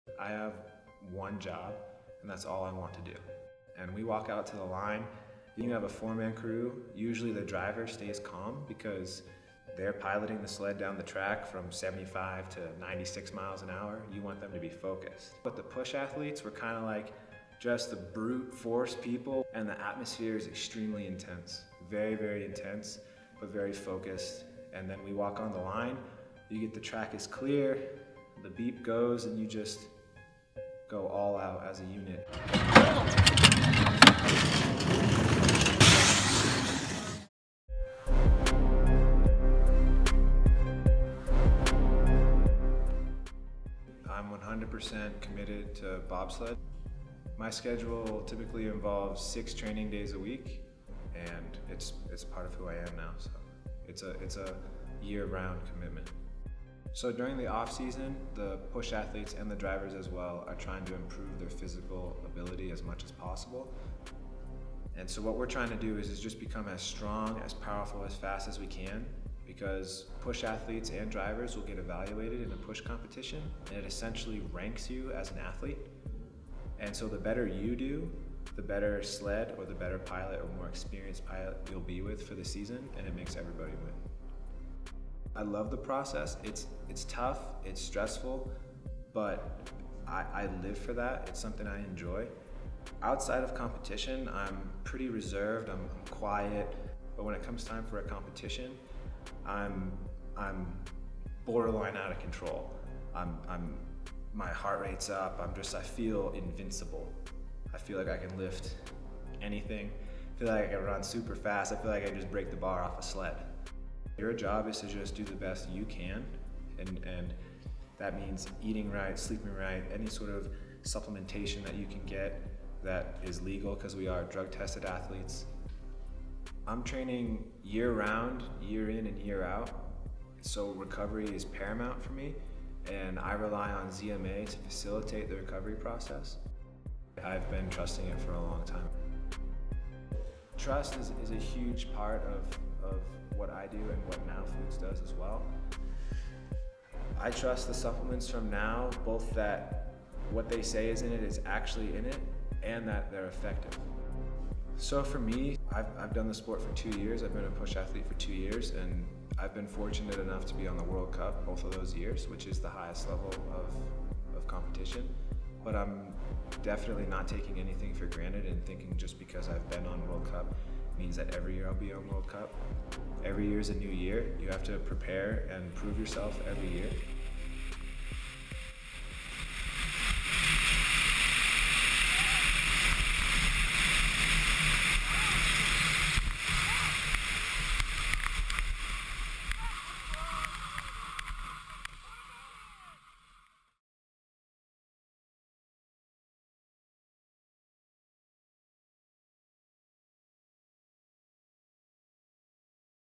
Frank DelDuca | U.S. National Bobsledder & NOW® Ambassador
U.S. National Bobsledder Frank DelDuca was a Division I track and field star whose collegiate career was cut short by an ACL injury. Instead of giving up on his dream, Frank went in an exciting new direction with bobsledding, and with the support of NOW he’s competing against the world’s best! Learn more about this inspiring athlete and how NOW supports his competitive journey